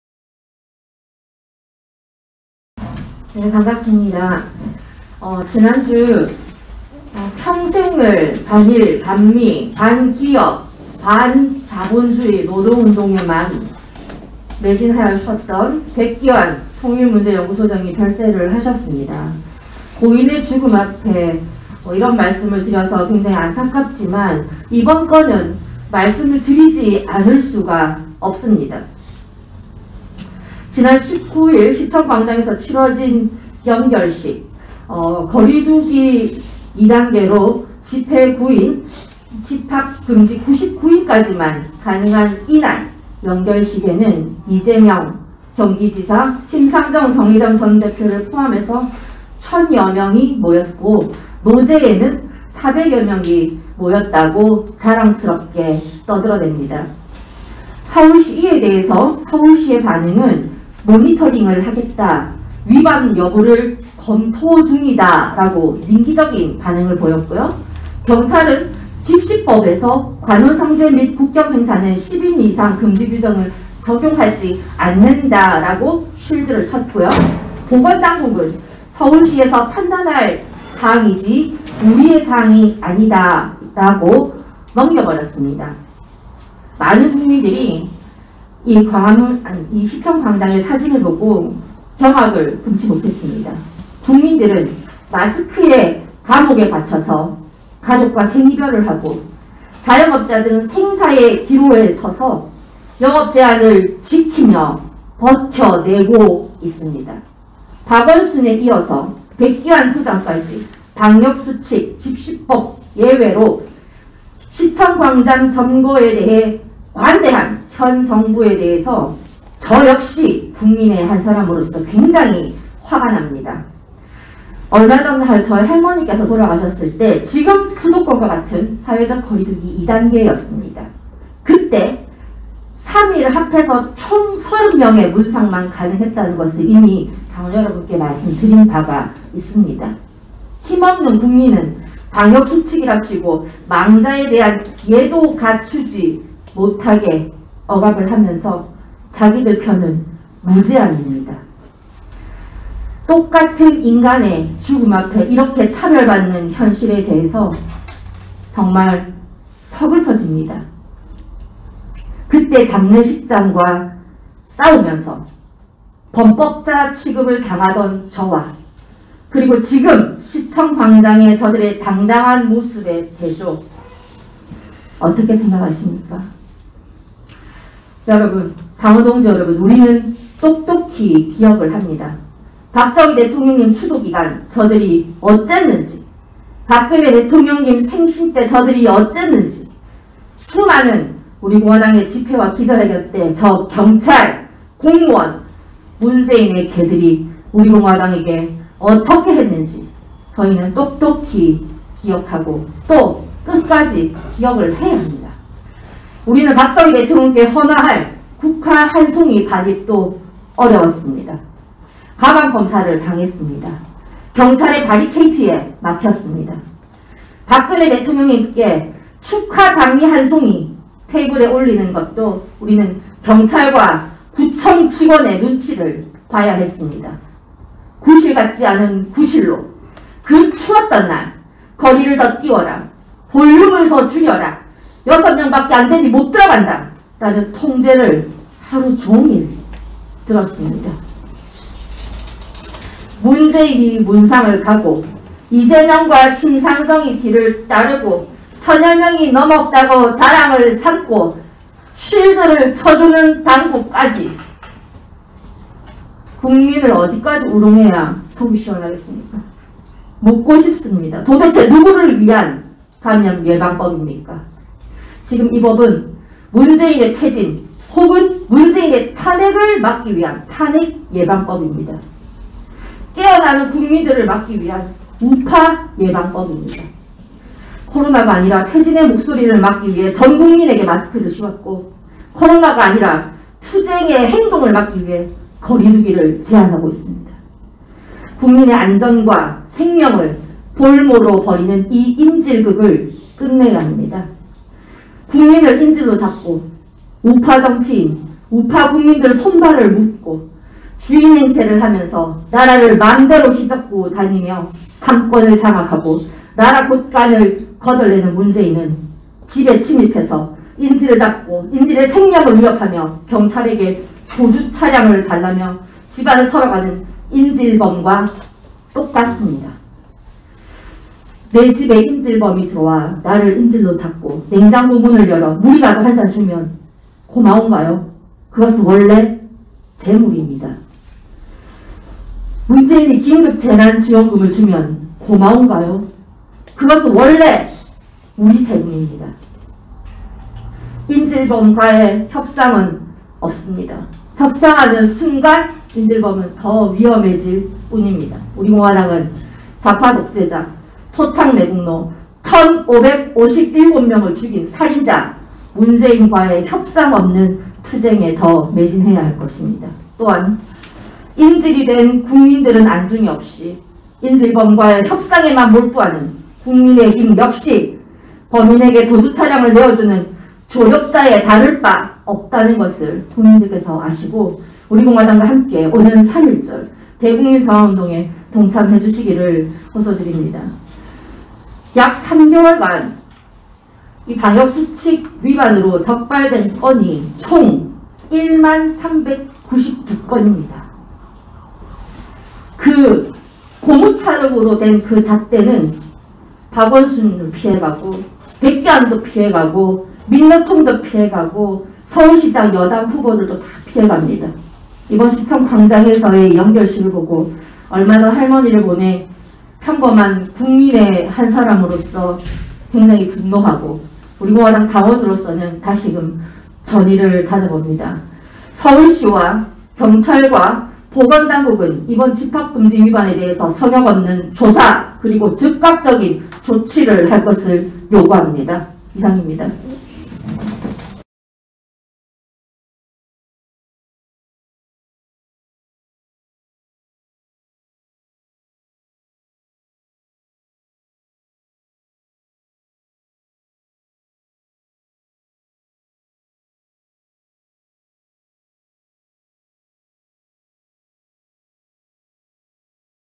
| 21.02.22 | 진순정 최고위원 발언! | 우리공화당 최고위원회의 | 2021년 2월 22일(월) |
👉출처 : 우리공화당LIVE  /
🔼일시 : 2021년 2월 22일(월)
🔼장소 : 우리공화당 중앙당사 대회의실
🔼 It's the Outsiders that change the world..!!

🔼 Freedom to President Park Geun-hye!!
🔼 Justice and Truth!!

🔼 Our Republican Party !